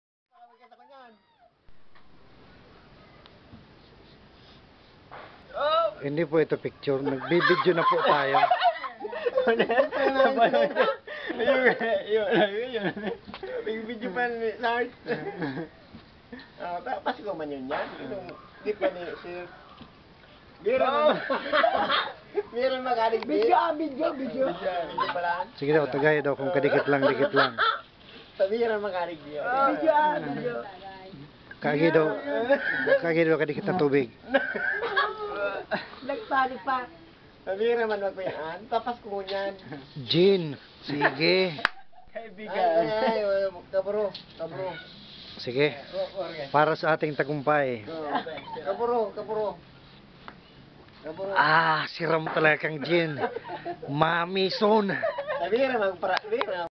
Gubat, Tigaon, Camarines Sur December 25, 2010 (HD) Ginebra San Miguel shot
A group celebrating Christmas over a bottle of Ginebra San Miguel asks Joseph Alan Cano to have a shot.